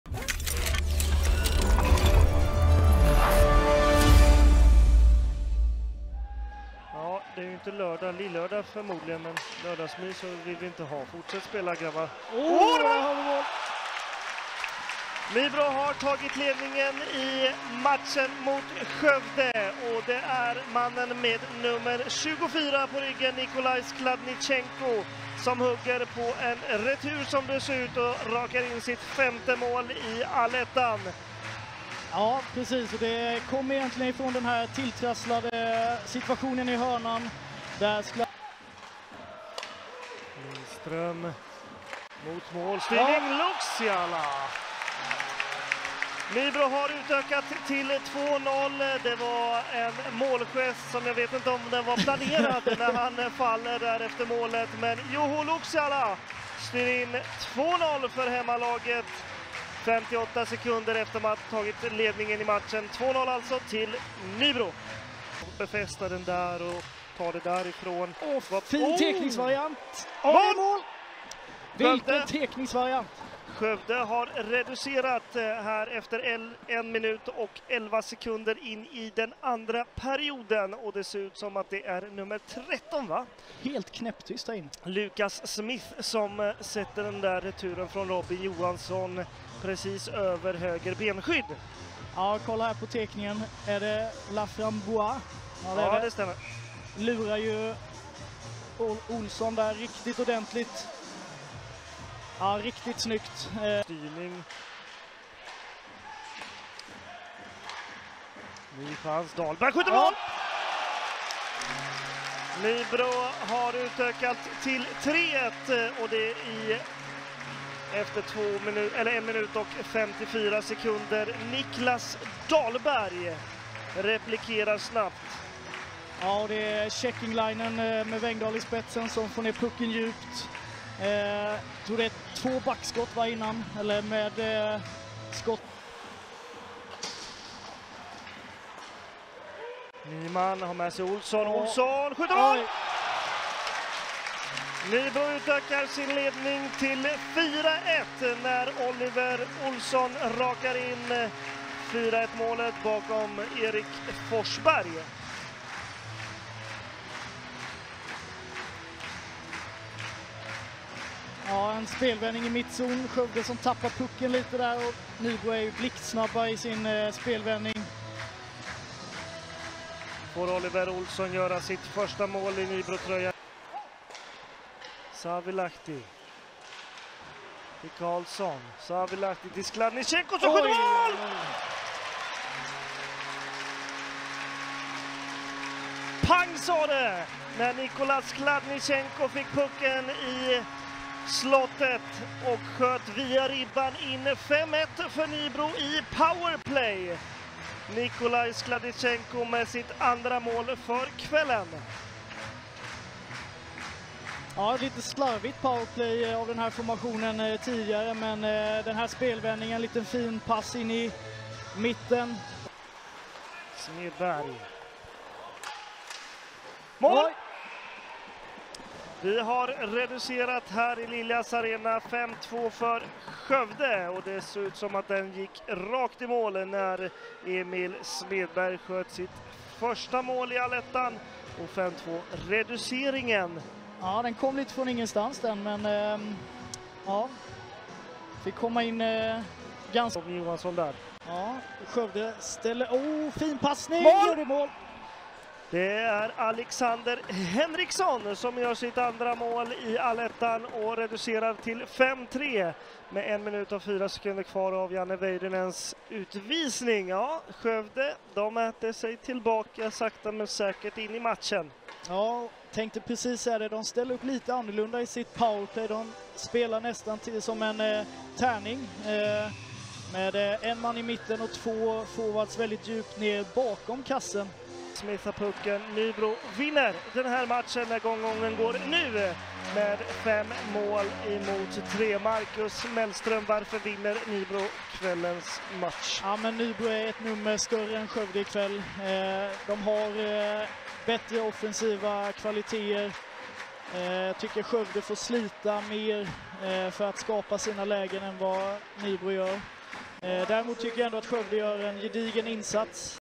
Highlights: Nybro Vikings - Skövde IK (5-3)
Nybro Vikings besegrar Skövde IK med 5-3 i Liljas Arena under onsdagskvällen. Nedan finns målskyttar och statistik från matchen.

Nybro Vikings – Skövde IK 5-3 (2–0, 3–2, 0-1)

Målen:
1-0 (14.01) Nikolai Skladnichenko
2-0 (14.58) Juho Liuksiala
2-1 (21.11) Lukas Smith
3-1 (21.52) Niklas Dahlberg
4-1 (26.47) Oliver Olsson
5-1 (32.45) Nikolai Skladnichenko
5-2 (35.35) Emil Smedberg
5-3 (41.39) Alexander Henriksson